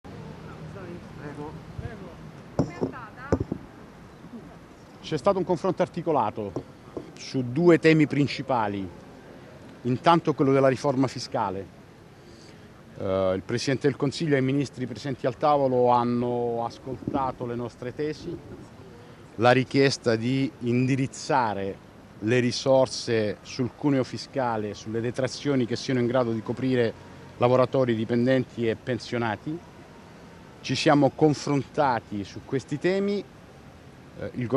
Bombardieri (Uil): "Draghi aperto al confronto su lavoratori e pensionati"
(Agenzia Vista) - Roma, 16 Novembre 2021 - "Un confronto articolato con il Presidente Draghi, sul tavolo lavoratori e pensionati. Il confronto è aperto e proseguirà nelle prossime settimane". Così il segretario generale Uil Pierpaolo Bombardieri dopo l'incontro a Palazzo Chigi con il Presidente del Consiglio Draghi.